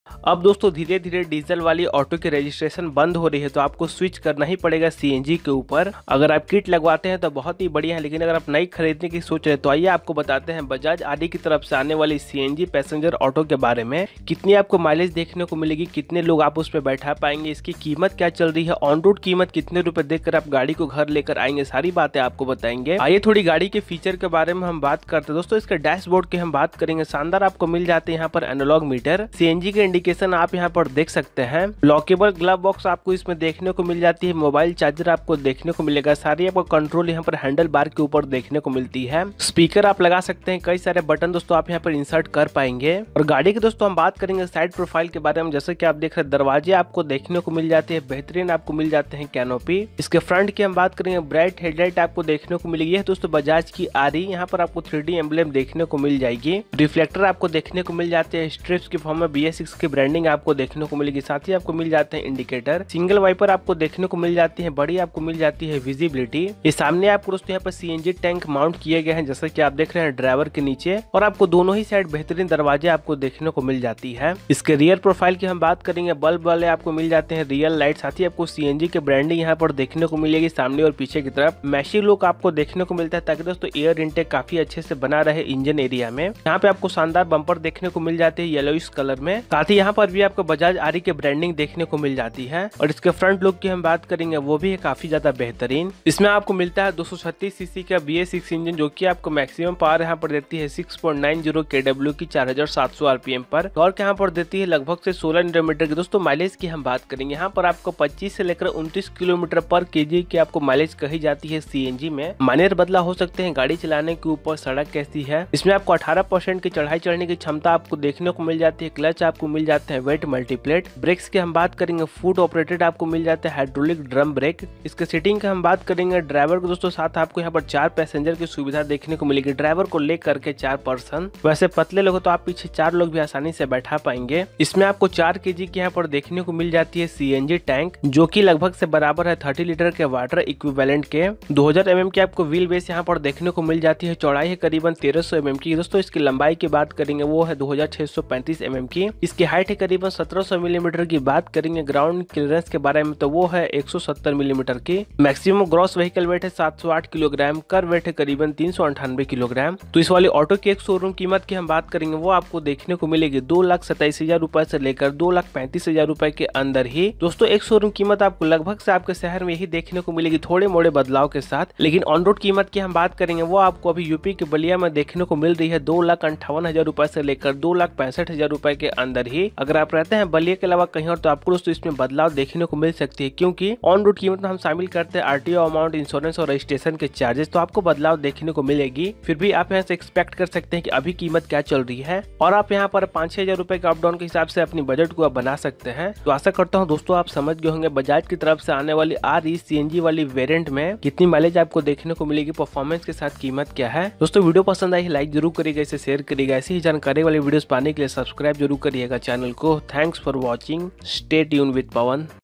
Bajaj RE CNG Auto Full Review | माइलेज 25+km bajaj re cng bs6 onroad price, Mileage, Features, Specs
3 seater passenger commercial auto bajaj re cng bs6 onroad price. Explained bajaj re cng auto full hindi review with colour mileage features specifications ki jankari.

🙄before buy cng auto jane deails of bajaj re cng bs6 & showroom price.

SOURCE - OWN and bajajauto officials